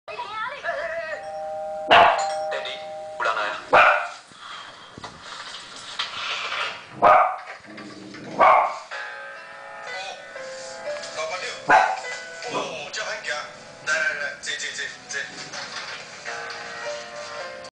Toby was Barking at the Door bell ring in the TV show - 2010.08
Toby (Dog) heard the the Door bell ring in the TV show, he was barking & jumped from the sofa to the Door. So Cute~